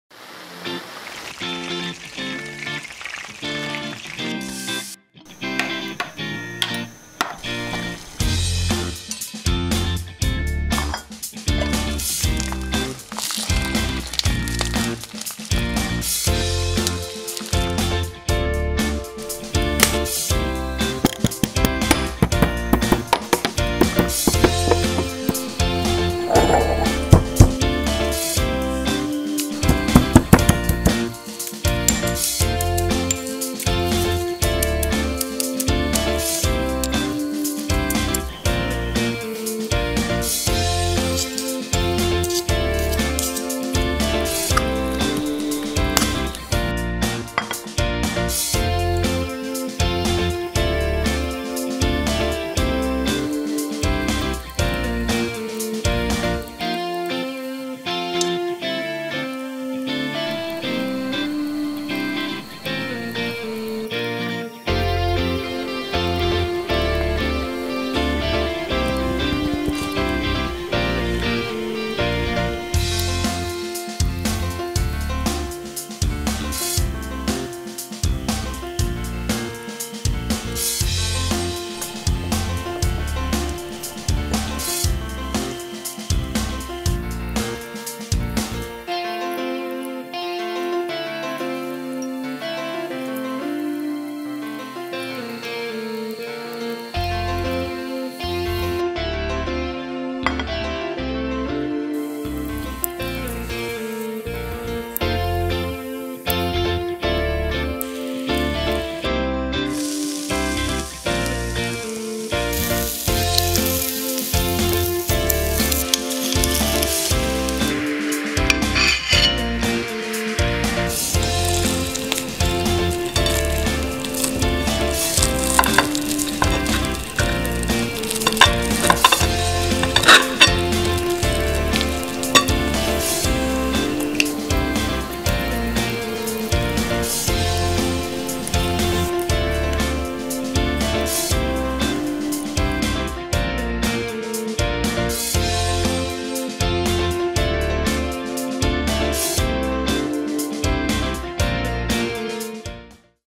Crispy and cheesy! Grilled chilli cheese sandwich!
Cheesy goodness for the entire family! More flavour, more deliciousness than any other grilled cheese sandwich!